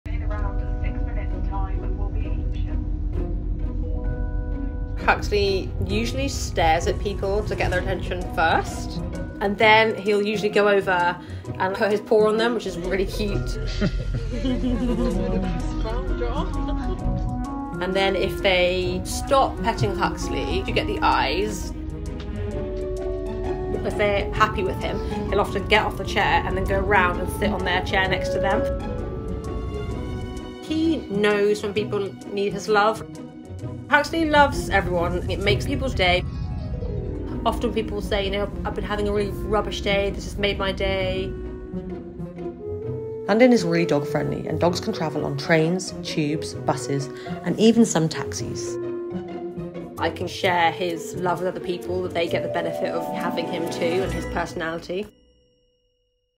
Dog Insists On Saying Hi To Everyone On His Train Rides | The Dodo
The best hello ever!

Follow @hugoandursula on Instagram.